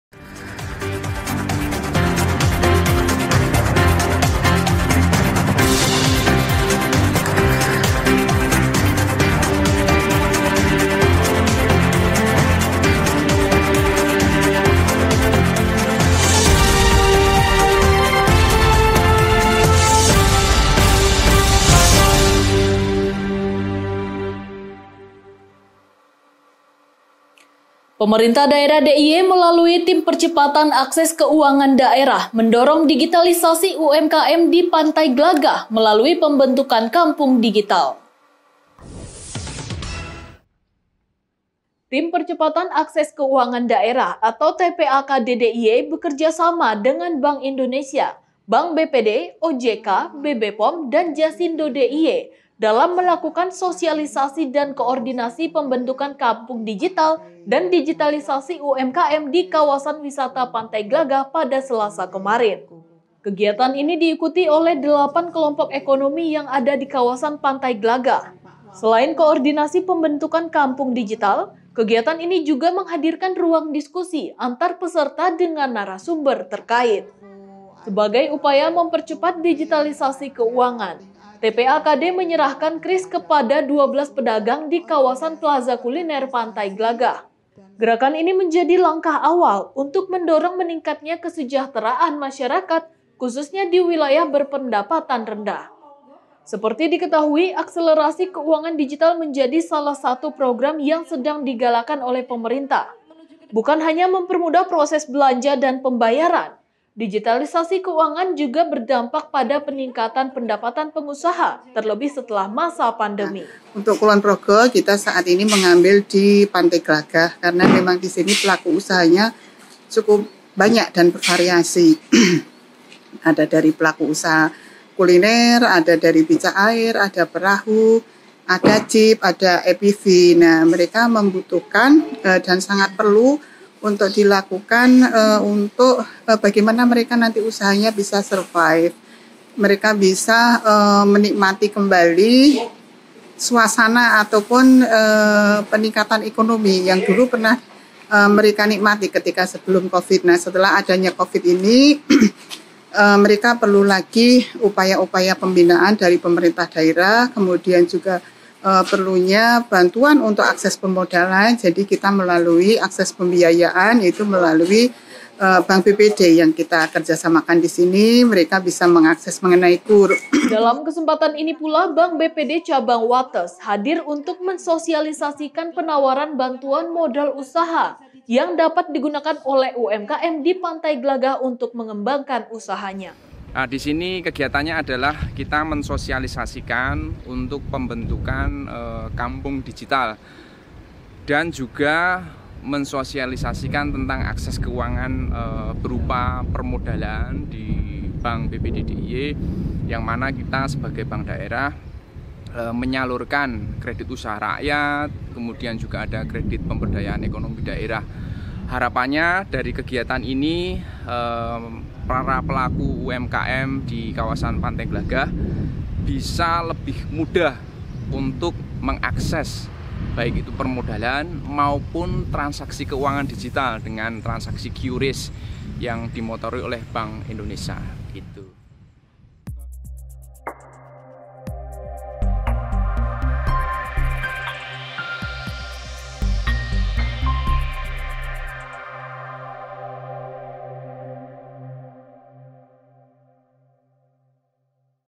PEMDA DIY BENTUK KAMPUNG DIGITAL GUNA MENDORONG DIGITALISASI UMKM - I JOGJA
TIM PERCEPATAN AKSES KEUANGAN DAERAH (TPAKD DIY BEKERJA SAMA DENGAN BANK INDONESIA, BANK BPD, OJK, BBPOM DAN JASINDO DIY DALAM MELAKUKAN SOSIALISASI DAN KOORDINASI PEMBENTUKAN KAMPUNG DIGITAL DAN DIGITALISASI UMKM DI KAWASAN WISATA PANTAI GLAGAH

✅ Mobile App "Jogja Istimewa"
✅ Channel "JITV Pemda DIY" di Youtube
✅ TV Kabel "Life Media" (JMN)
✅ Layanan Streaming Genflix.co.id dengan Keyword "JITV"